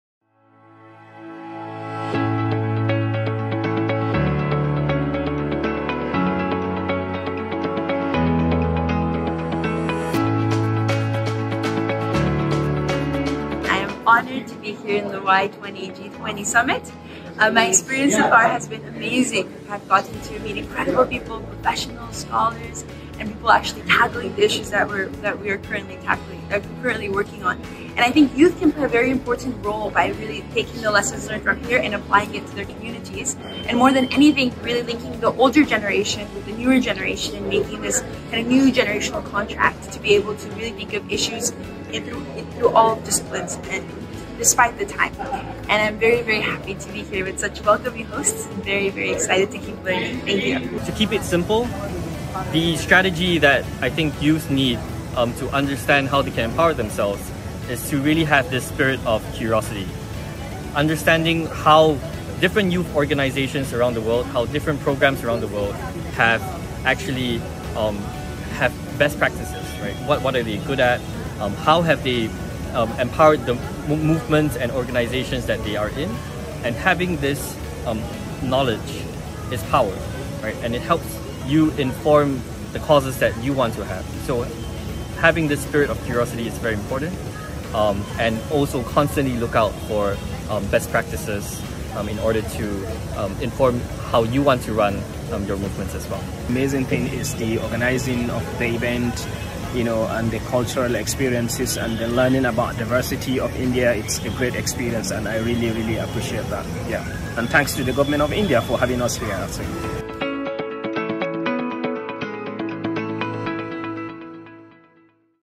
Youth20 Summit in Varanasi
Delegates come together to share best practices at the Youth 20 Summit in Varanasi